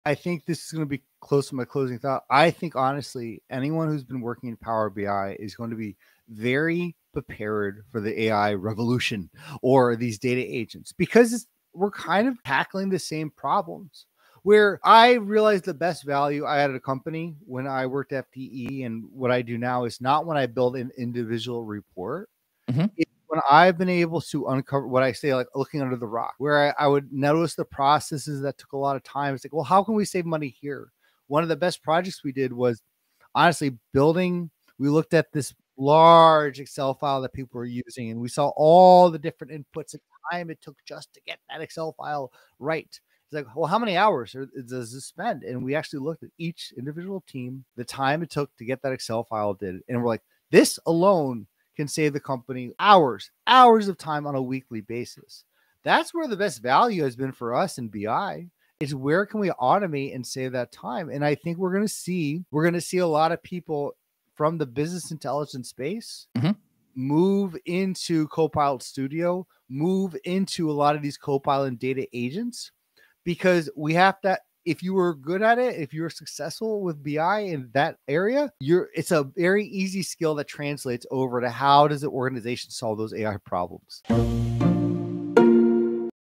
477: Power BI Pro's Secret: AI Agents Automate & Save You Time!
Power BI pros are uniquely prepared for the AI revolution. They excel at uncovering hidden process inefficiencies and automating tasks—saving companies countless hours and dollars. The AI future is now!